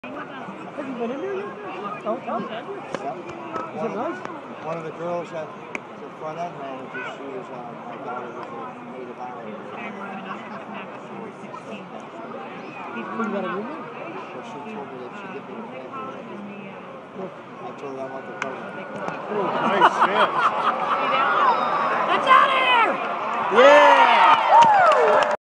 Buffalo bisons baseball
Buffalo bisons beat Pawtucket red socks 15-3